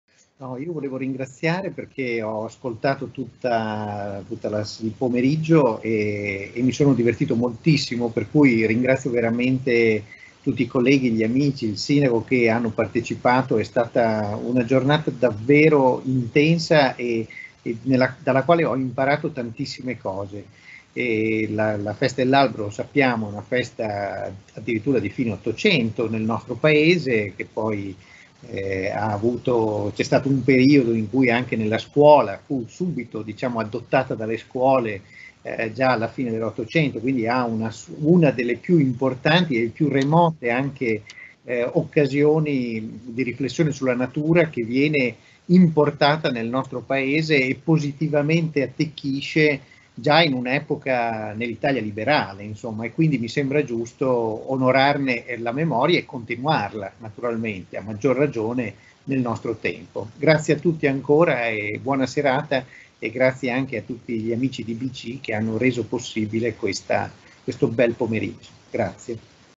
Conclusioni di Roberto Balzani, presidente IBC
Conclusioni di Roberto Balzani, presidente dell'Istituto per i Beni Artistici, Culturali e Naturali della Regione Emilia-Romagna al convegno "Giardini, alberi...e noi. Cambia il rapporto tra uomo e natura?"
20 novembre 2020, nell'ambito di ViVi il Verde 2020